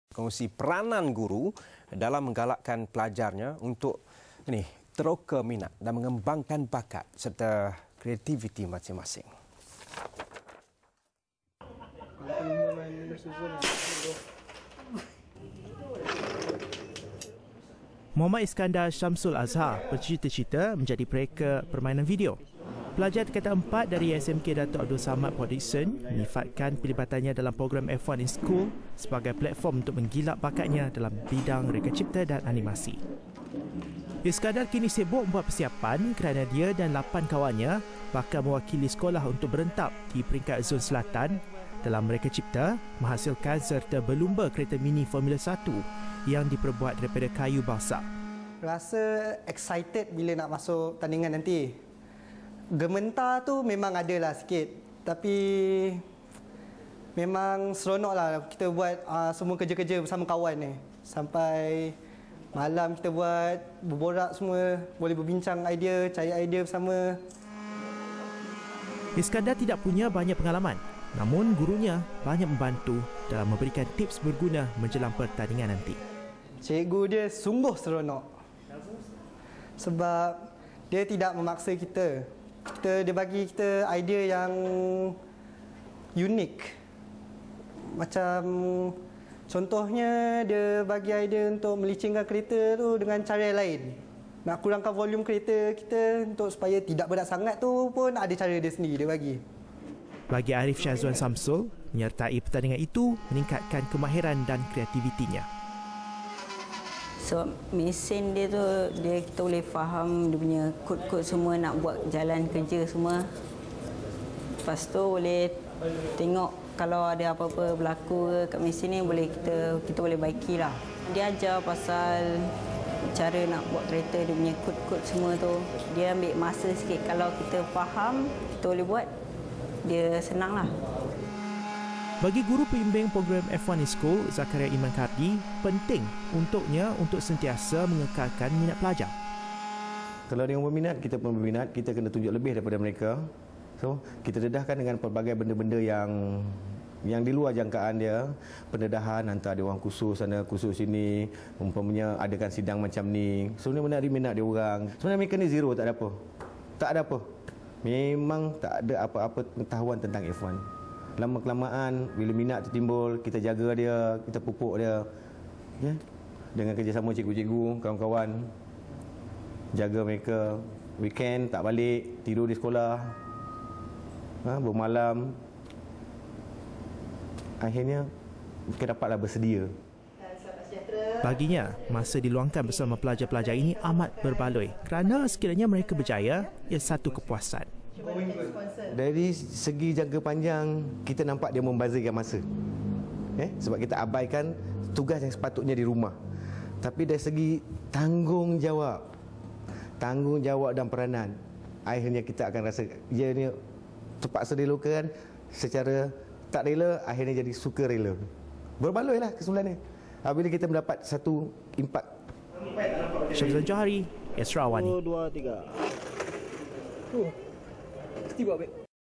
Guru serlah potensi pelajar melalui inovasi
Wartawan Astro Awani Shafizan Johari berkongsi peranan guru dalam menggalakkan pelajarnya untuk meneroka minat dan mengembangkan bakat serta kreativiti masing-masing.